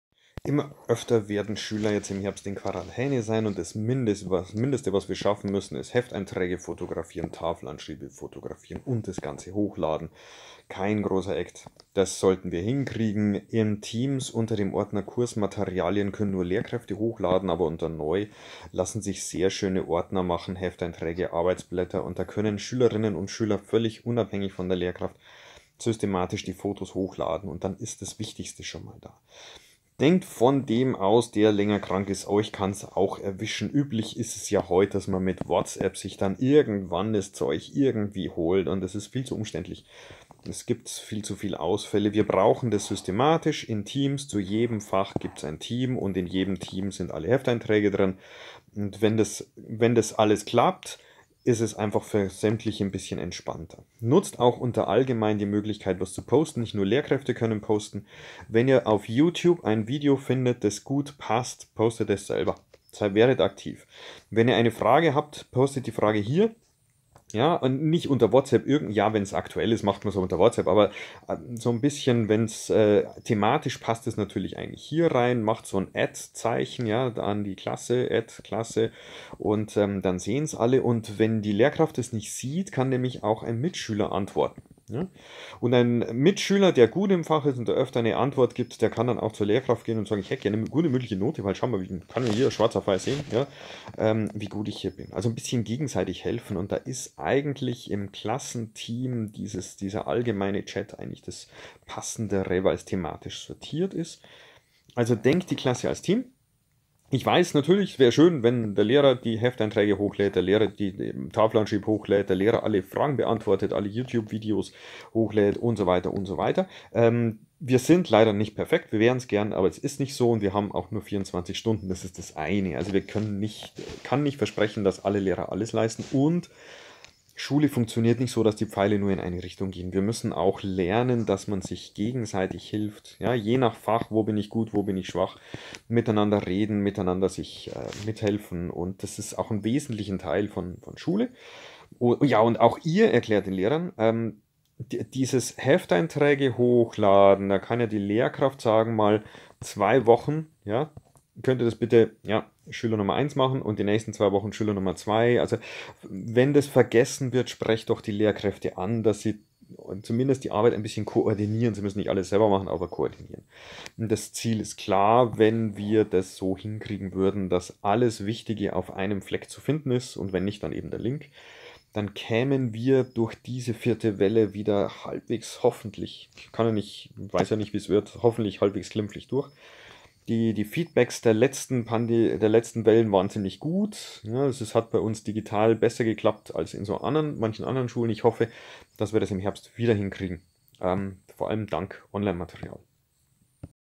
für AWG-Schüler:innen: Wir laden das Material bei Teams hoch.
Fotos von Hefteinträgen sind schnell gemacht, auch schnell hochgeladen. Wenn die Materialien alle bei Teams zu finden sind wird  es für Quarantäne-Schüler:innen viel leichter.